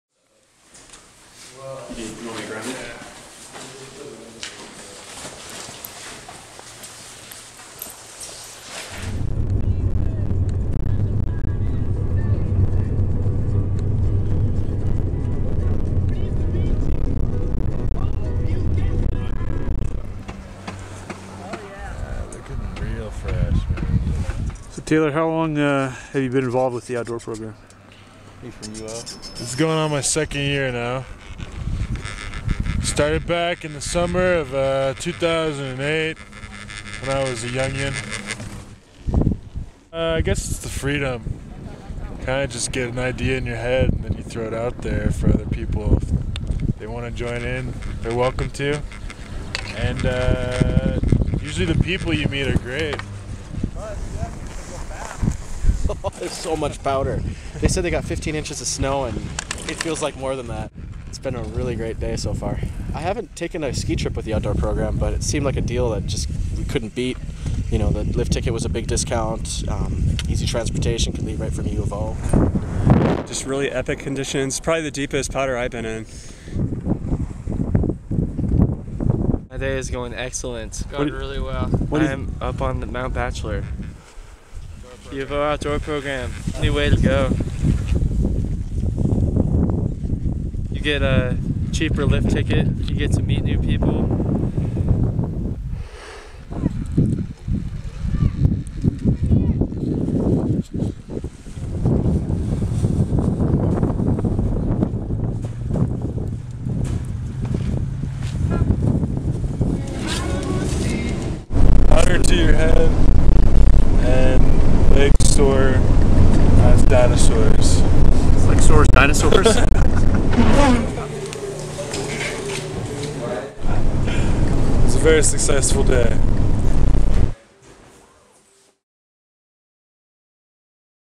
Mount Bachelor and the Outdoor Program
The University of Oregon Outdoor Program takes a trip to Mount Bachelor in central Oregon.